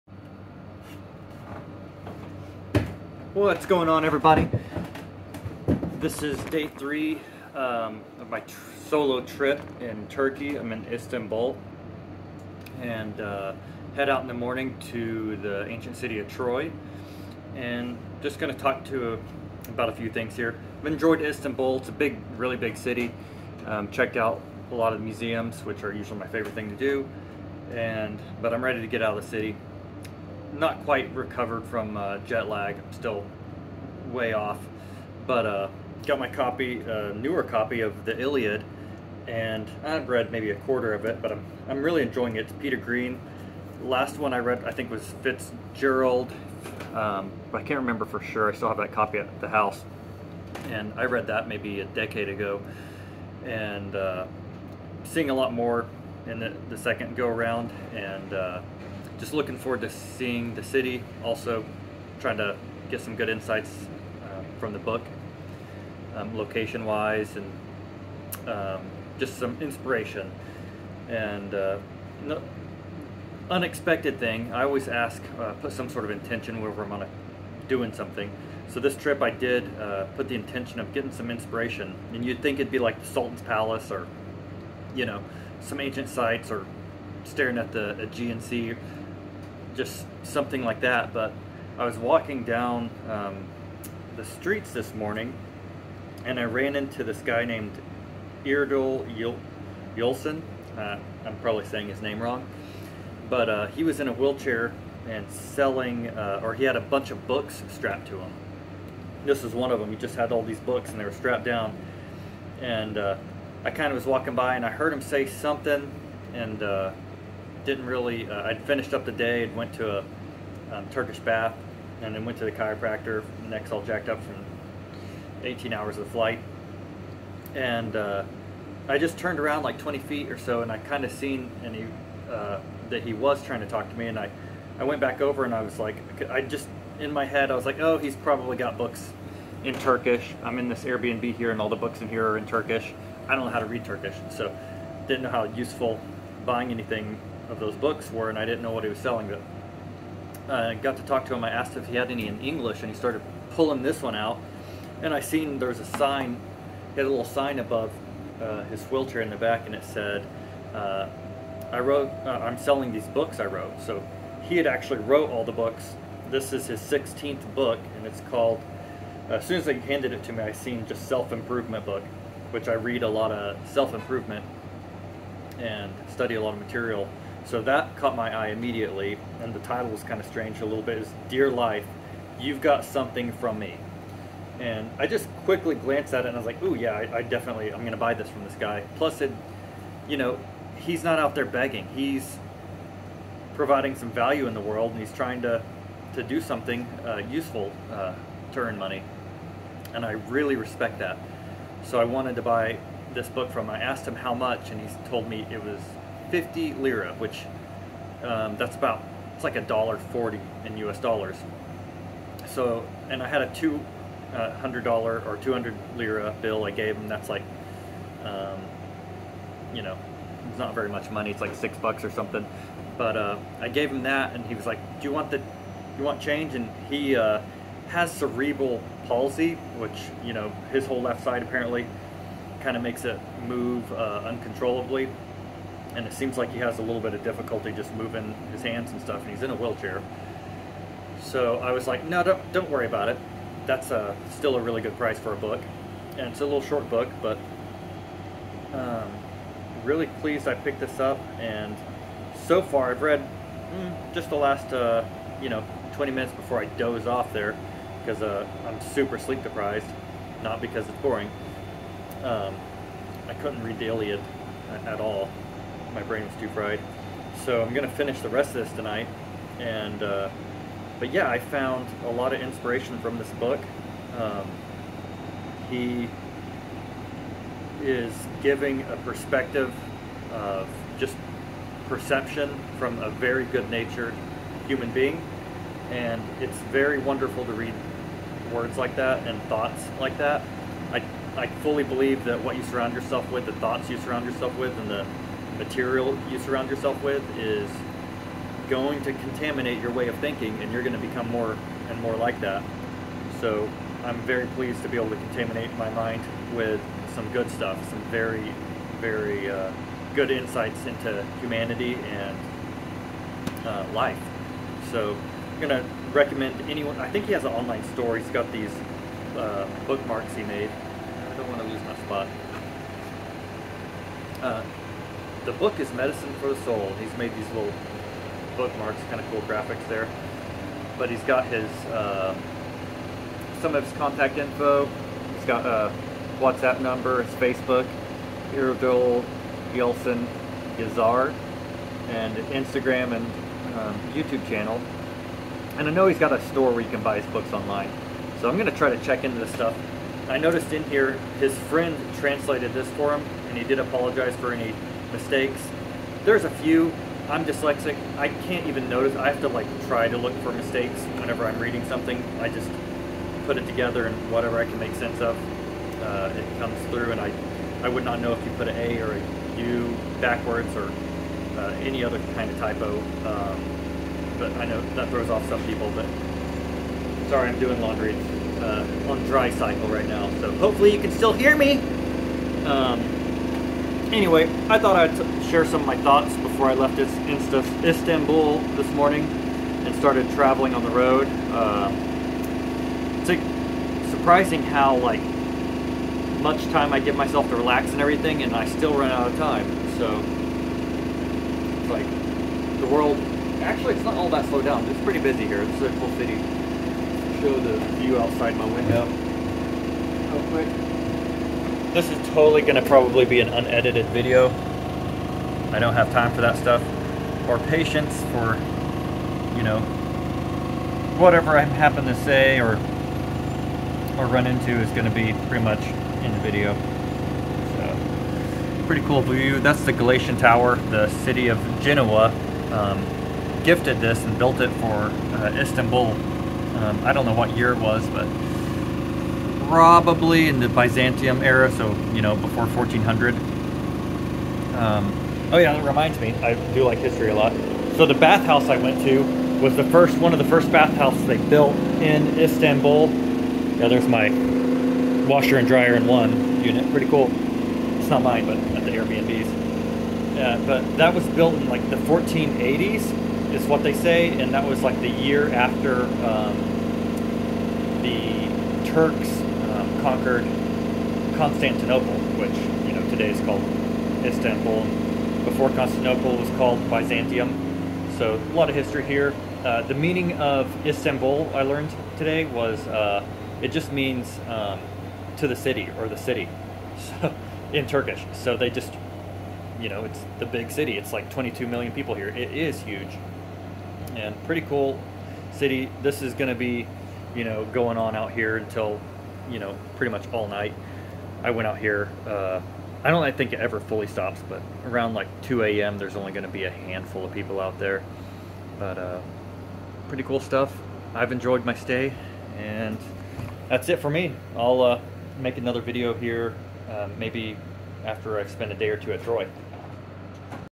Solo Trip to Turkey Day 3 in Istanbul - Inspired by disabled man selling his books
Solo trip in Turkey. The first 3 days spent in Istanbul. Some of my thoughts on the city and finding inspiration from a disabled man I met while walking on the street who was selling some books. he wrote. They were strapped to him since he had cerebral palsy. Yazar Erdal Yalcin, who has written at least 16 books with cerebral palsy.

Heading to see the ancient city of Troy tomorrow. I brought my copy of the Iliad reading a new translation  Peter Green. More thoughts on that in a later video?

(Just use Google translate to switch it over to English if you can't read Turkish)